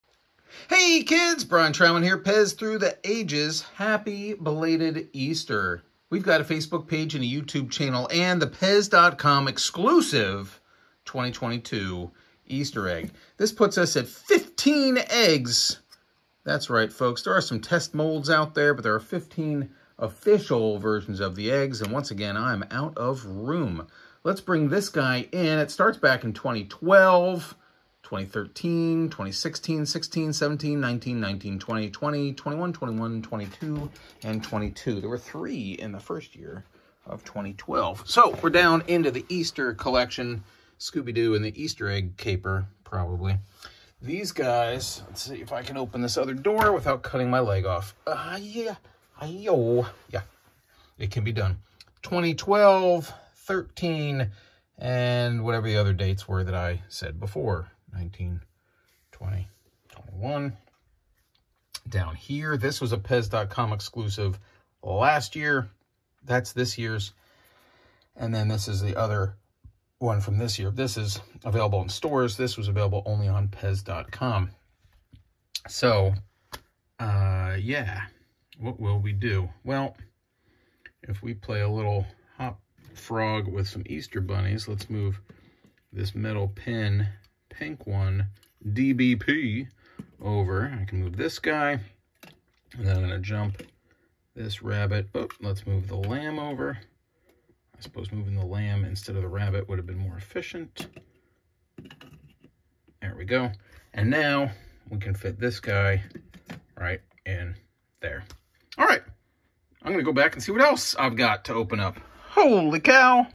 PEZ-exclusive Easter Egg 2022! Did you find one before the Easter Bunny found you and ate you?
Is that how Easter Egg hunts works?  It should be. A lot more thrilling if an angry bunny was chasing you.

Anyhow, Easter eggs are about rebirth, a new  life. I hope this PEZ-exclusive (not sold in stores) gives you new life!  Or at least gives you a few candies.